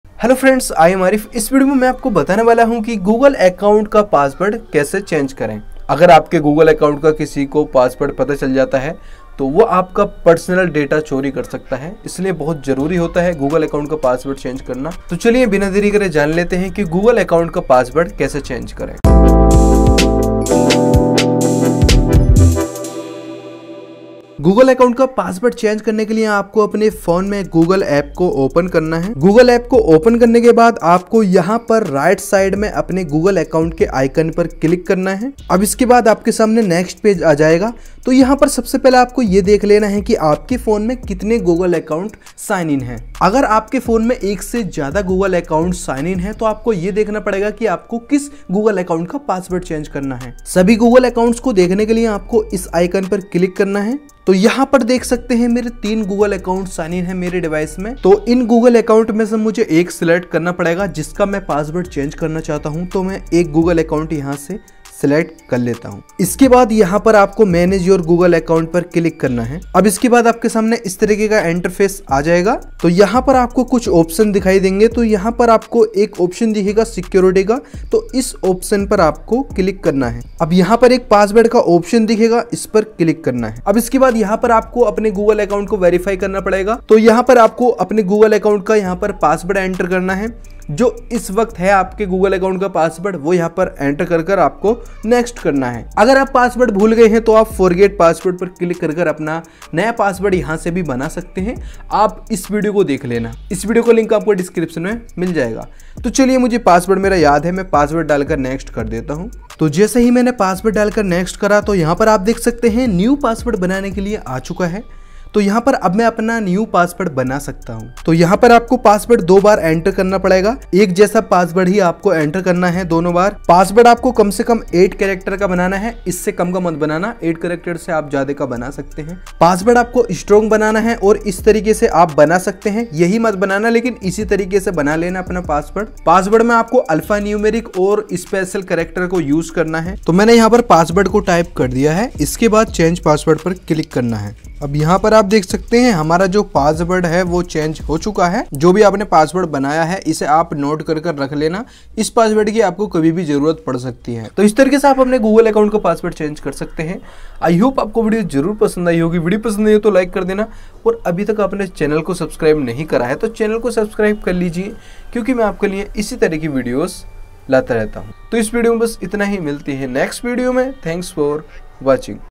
Google Account ka password Change Kaise kare | How To Change Google Account Password
About The Video:- Is Video mein bataya gya hai ki Google Account ka password kaise change karte hain.

About Me:- My name is Aarif Qureshi. I am from UP (BULANDSHAHR).